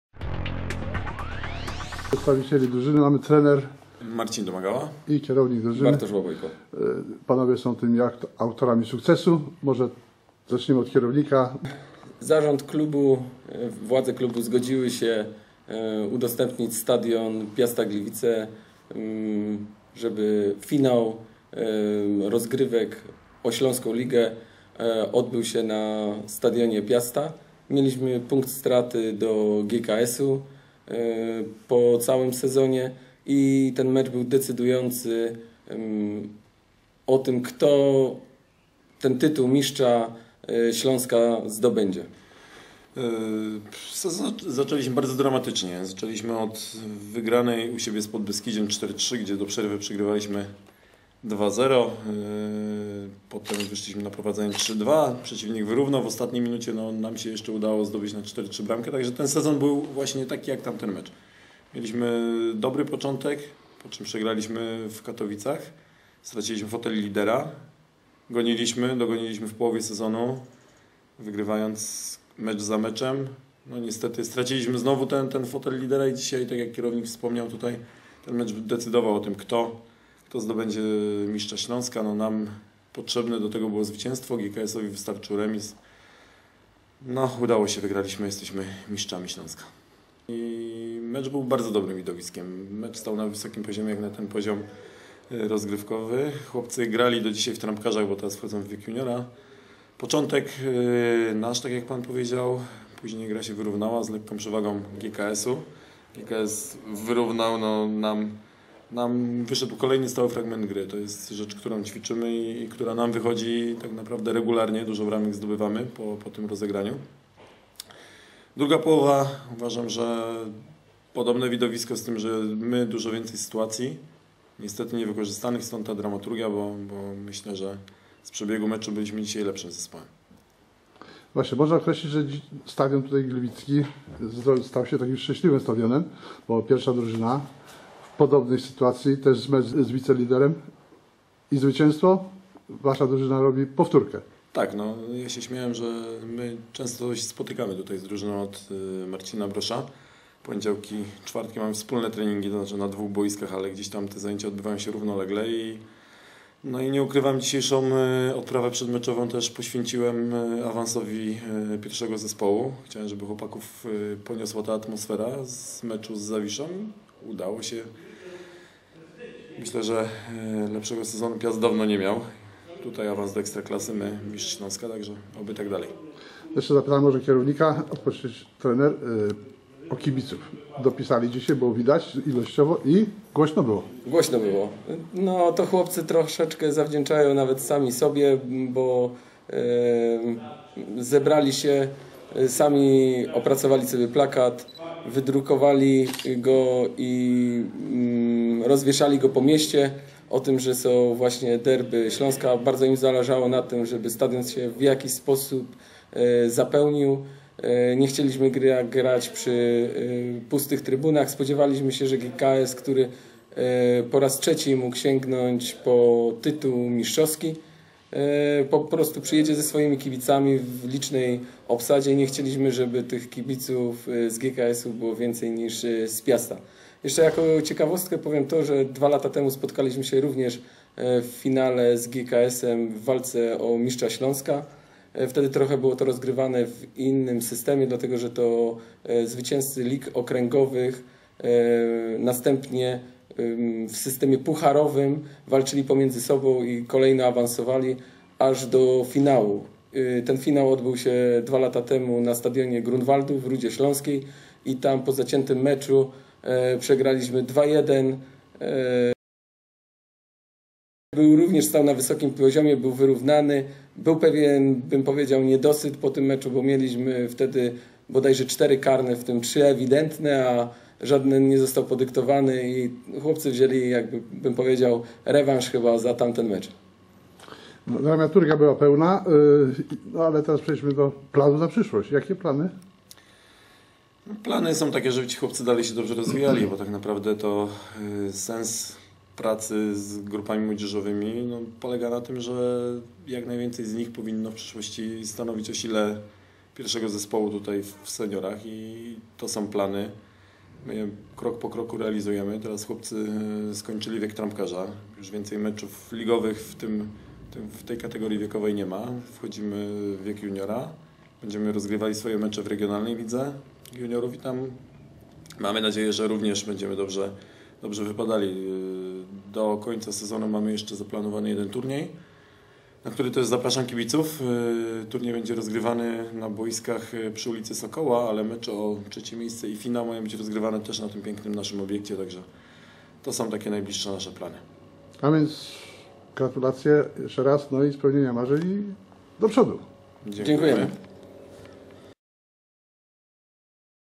ŚLT97 wypowiedzi po meczu na szczycie - 10.06.2012r.
Trener zespołu Piasta Gliwice z rocznika 1997 Marcin Domagała
i kierownik zespołu Bartłomiej Łabojko o meczu na szczycie z GKS-em Katowice i nie tylko.